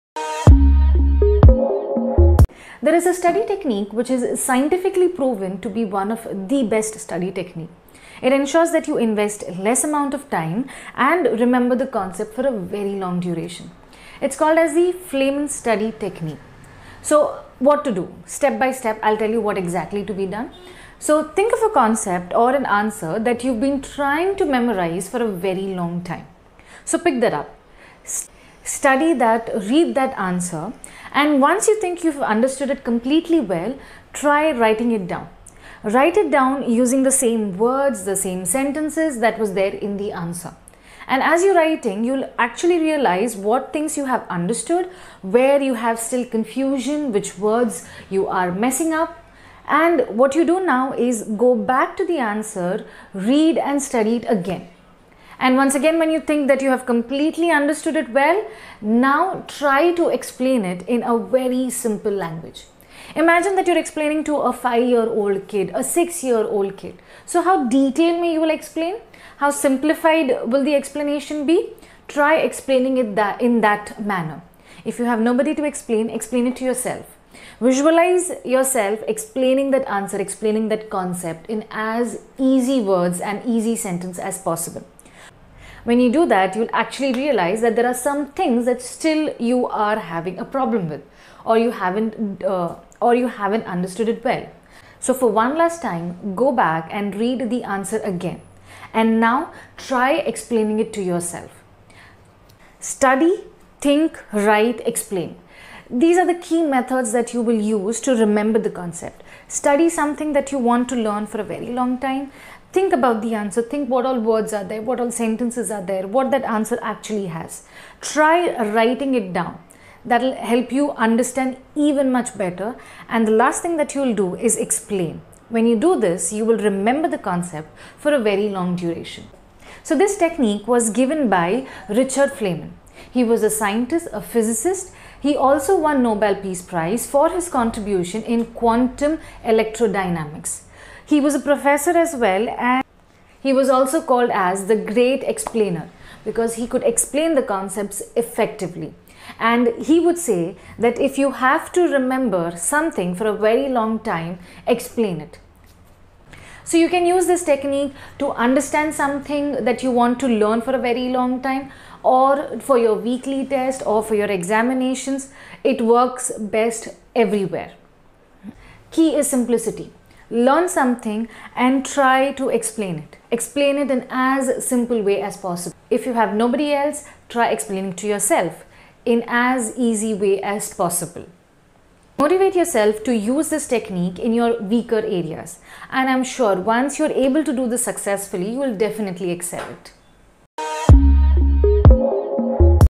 How to learn faster with the Feynman Study Technique!
What is the Feynman Study Technique? We find out!